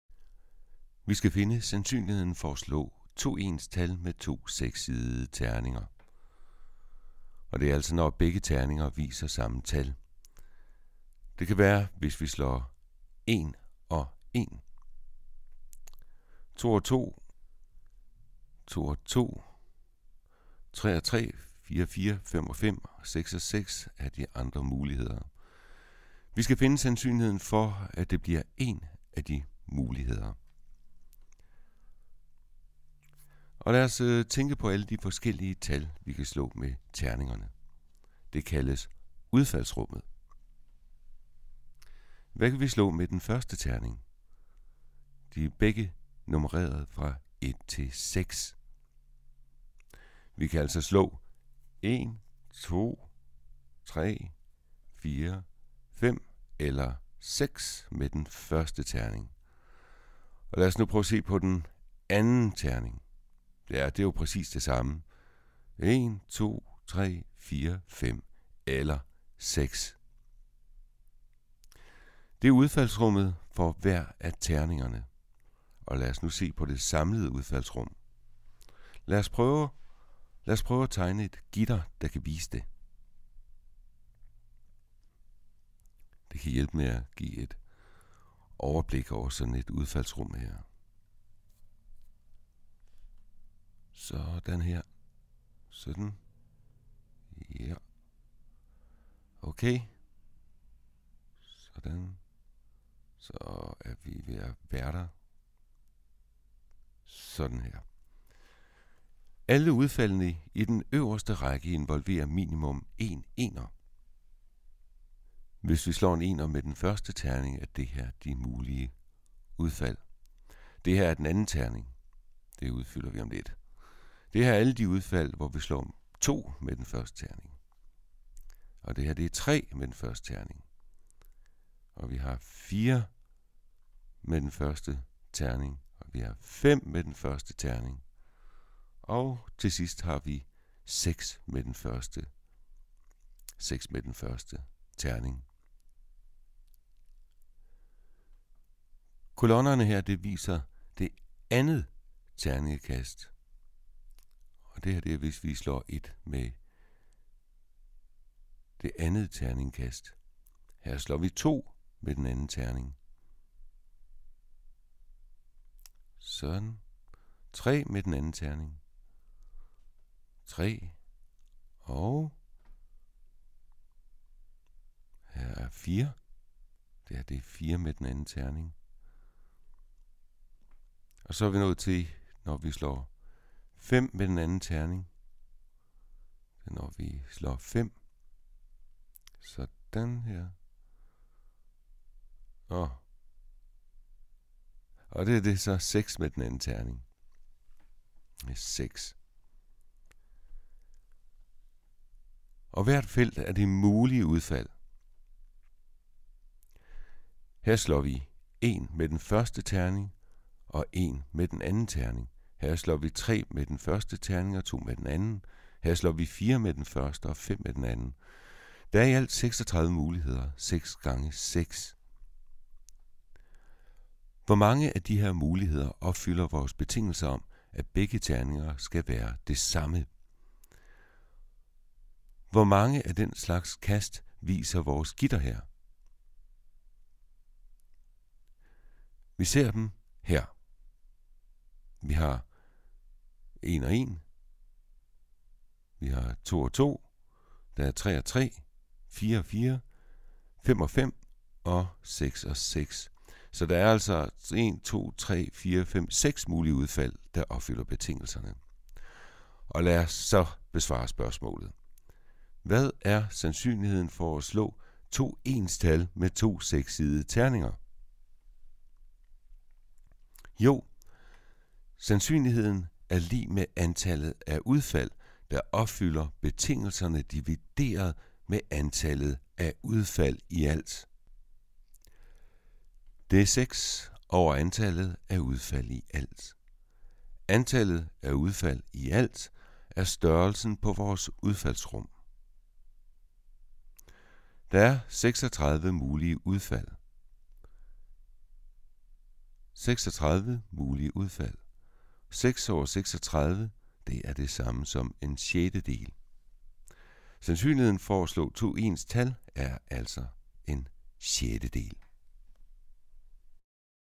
Sandsynlighed med terninger
Original titel: "Die rolling probability"

Sandsynligheden for at slå 2 ens tal med 2 terninger.